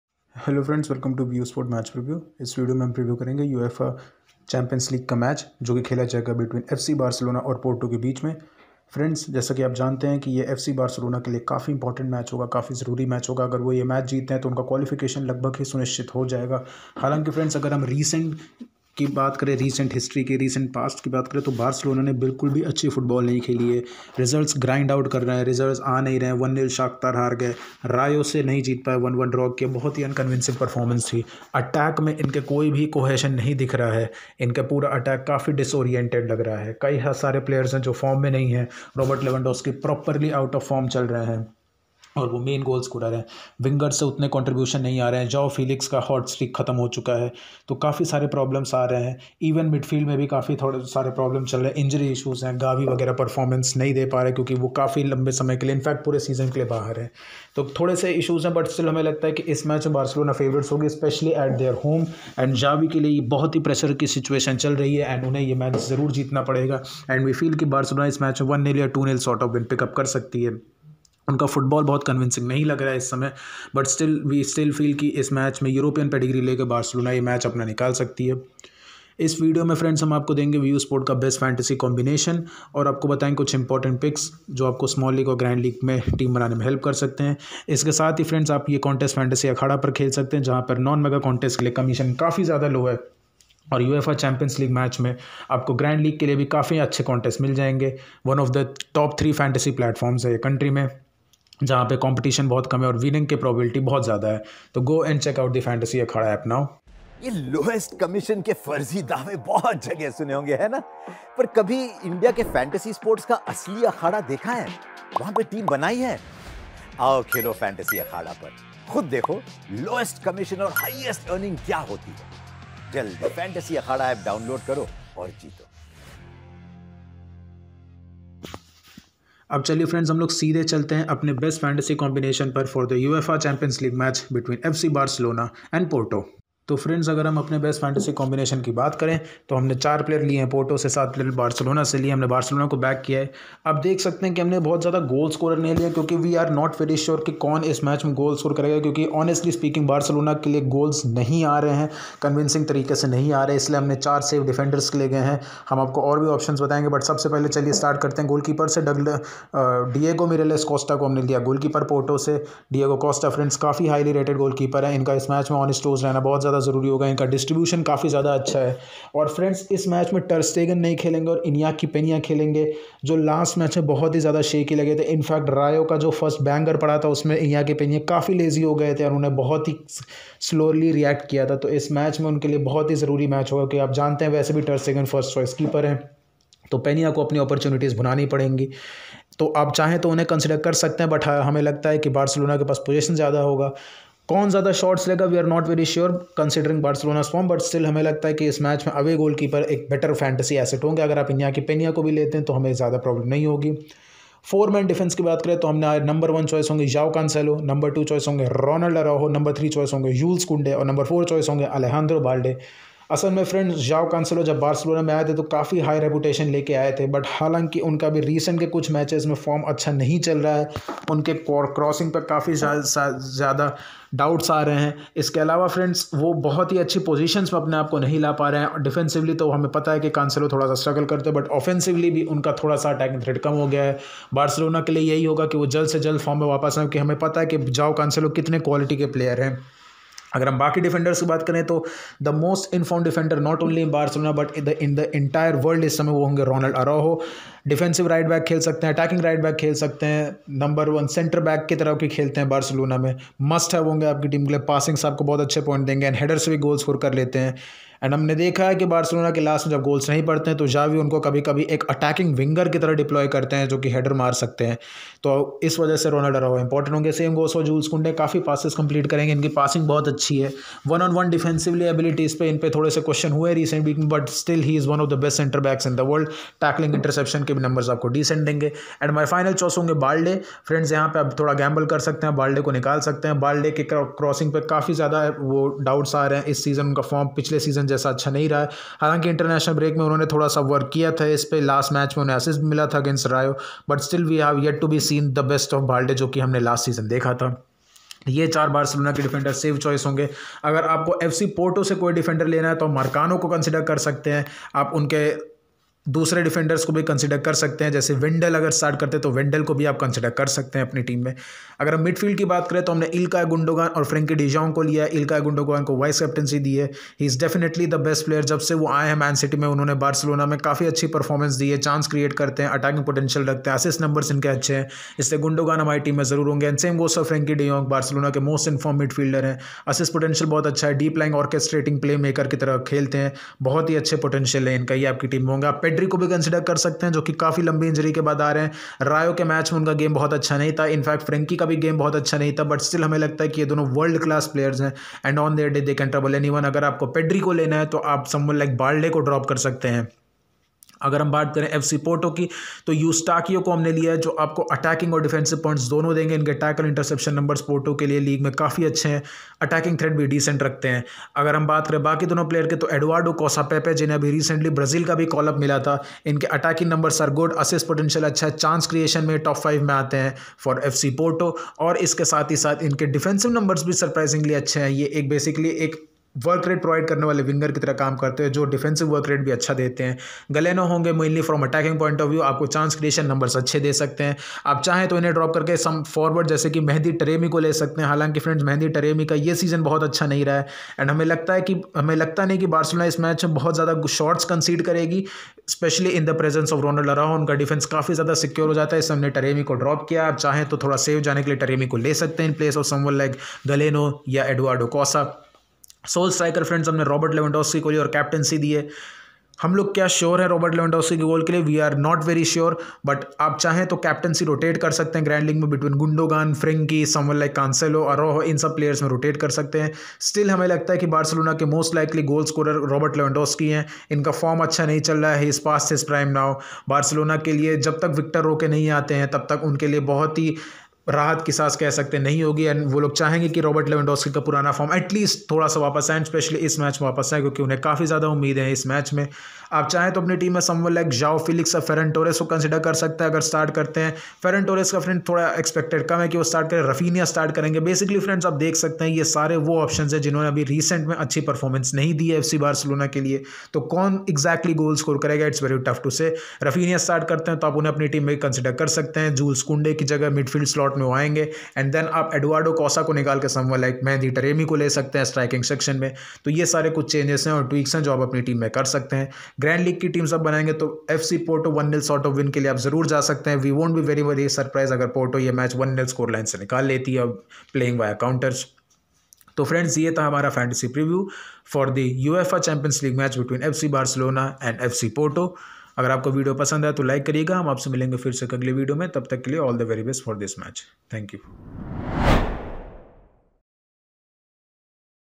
BAR vs POT Dream11 Prediction | Barcelona vs FC Porto | Champions League Dream 11 Preview
Get ready for the most awaited Champions League clash between Barcelona vs FC Porto.

In this video, we provide a detailed match preview of the BAR vs POT game, including our predictions, analysis, and insights. We also discuss the probable lineups, player performance, and tactical analysis to give you a complete picture of the upcoming fixture.